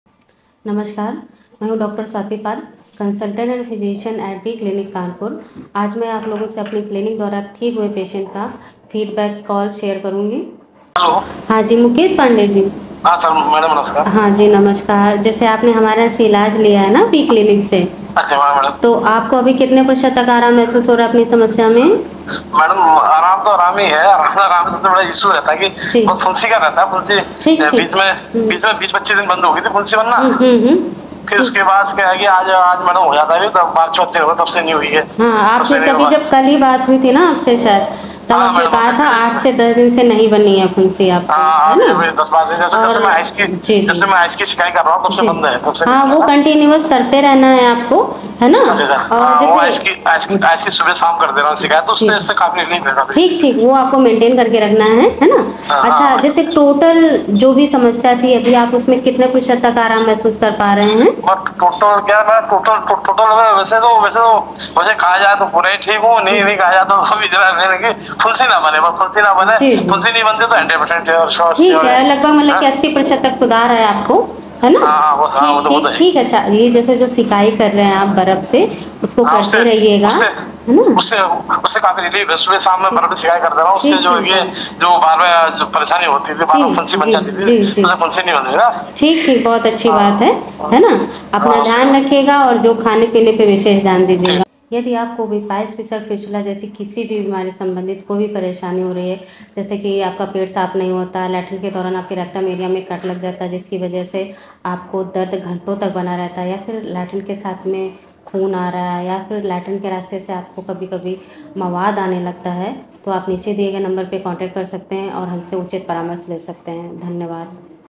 Fistula ka homeopathy Ilaj - Mukesh Pandey Ji ki Success Story | 191229
WeClinic™ के ऑनलाइनहोम्योपैथीकंसल्टेशन से फिस्टुला की समस्या से सही हुए Mukesh Pandey Ji का फीडबैक
Mukesh Pandey Ji गैस, पेट ना साफ होना , पस आने एवं खून की समस्या से 2 /वर्षो से परेशान थे।

Mukesh Pandey Ji ने WeClinic™ में दिनांक 15/07/2024 को पहली बार ऑनलाइन दवा बुक करवाई, उस समय Mukesh Pandey Ji को गैस , कब्ज और मस्सो की समस्या थी।

Mukesh Pandey Ji ने कुल 2 महीने WeClinic™ में अपना इलाज चलाया, आज इनको Mukesh Pandey Ji 80 %आराम है

WeClinic™ ke OnlineHomeopathyConsultation se Fistula ki samasya se sahi huye Mukesh Pandey Ji ka feedback

Mukesh Pandey Ji 2 /varsho se Fistula ki samasya se pareshan the.

Mukesh Pandey Ji ne WeClinic™ mein dinank 15/07/2024 ko pehli baar online dawa book karwayi, us samay inko gas, kabj,pet na saf hona, pus aana evam khun aane ki samasya thi.

Mukesh Pandey Ji ne kul 2
mahine WeClinic™ mein apna ilaj chalaya, Mukesh Pandey Ji ko gas, kabj,pet na saf hona, pus aana evam khun aane ki samsya me 80%aaram hai.

Agar aap fistula ki samasya se pareshan hain to aaj hi apna ilaj WeClinic™ se shuru karein

Feedback from a patient who successfully recovered from Fistulaissues through onlinehomeopathyconsultation from Weclinic

Mukesh Pandey Ji, had been dealing with issues related to gas, incomplete bowel movements, pus formation, and bleeding for 2 /years.

Mukesh Pandey Ji initiated their online medicine consultation with WeClinic™ on 15/07/2024, reporting issues such as gas, constipation, and masses at that time.

Over the course of 2 months, Mukesh Pandey Ji underwent treatment with WeClinic™. Today, Mukesh Pandey Ji experiences relief from gas, constipation, and masses a 80 %improvement in their condition.

If you are troubled by fistula issues, start your treatment with WeClinic™ today.